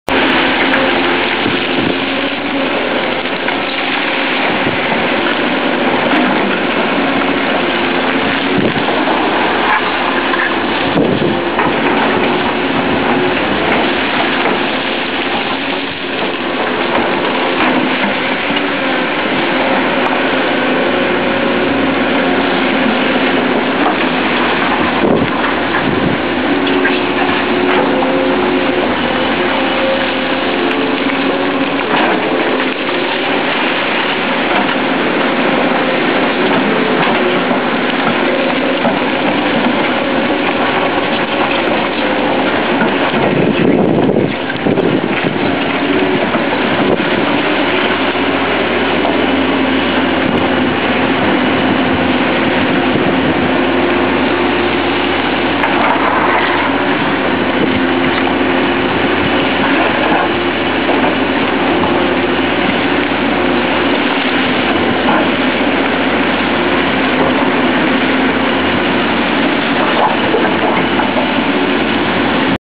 Excavators Cat 325D & Cat 323D Move Big Rocks
Caterpillar 325D & 323D , costruzione scogliera tagliamento